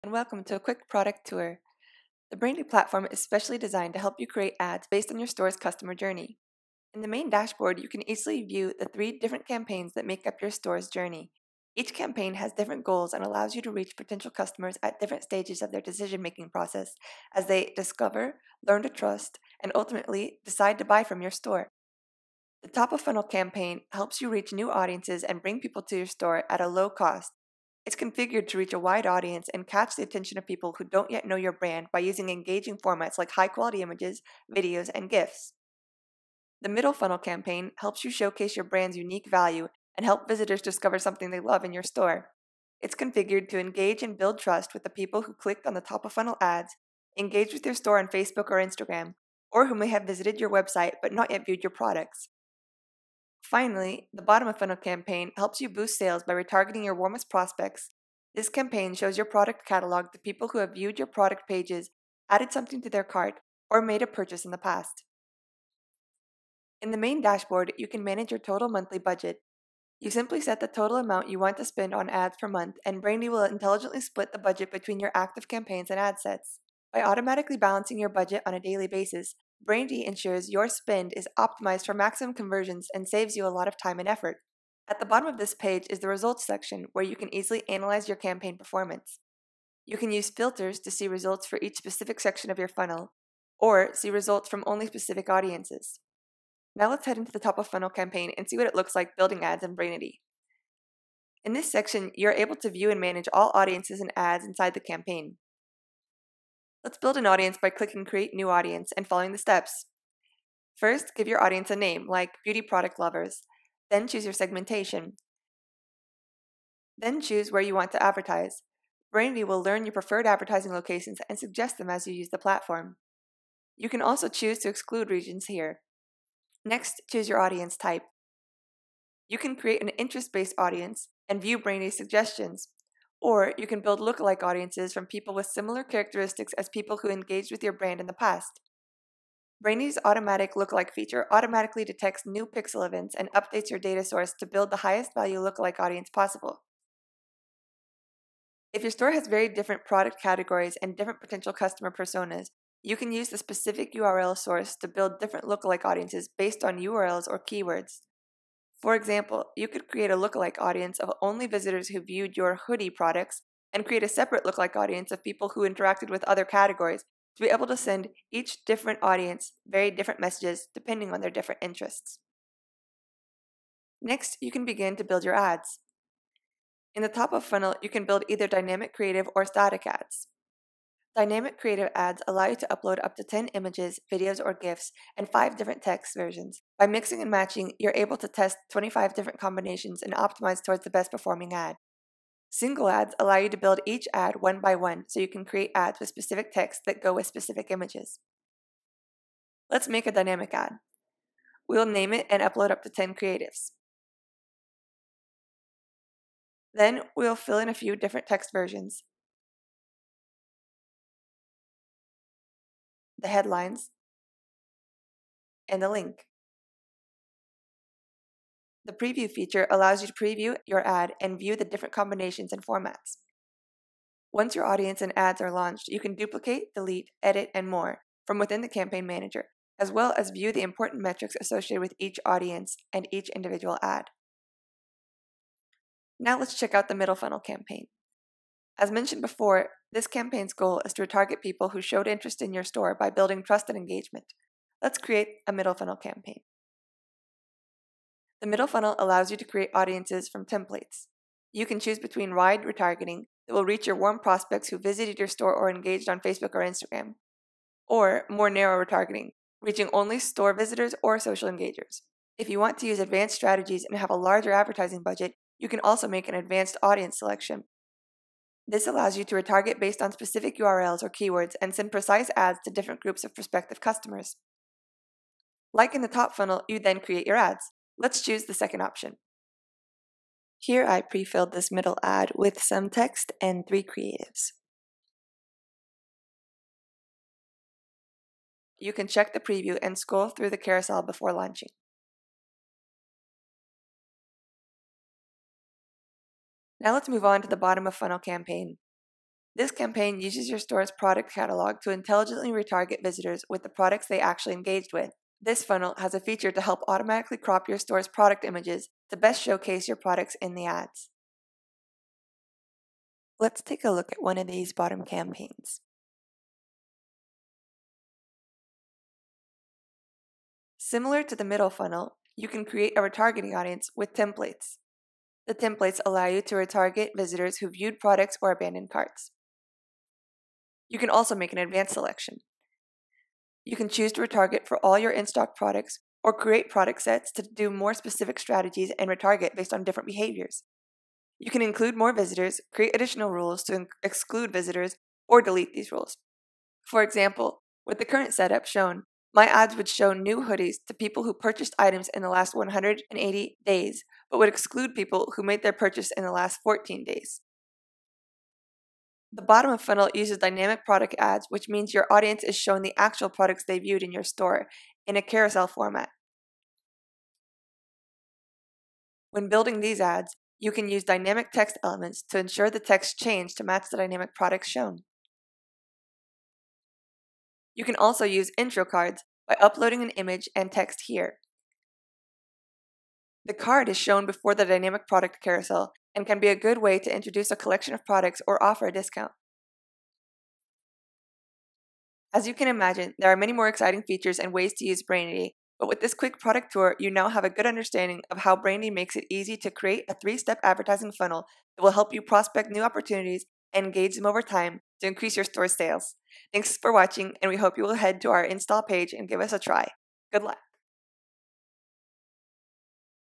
Brainity Product Tour – Automated Customer Journey ads for Facebook and Instagram
This product tour shares an overview of Brainity's main features. With a quick look into a demo store, we show how easy it is to build automated customer journey ads on Facebook and Instagram and share some insight into how Brainity can be used to execute advanced advertising strategies.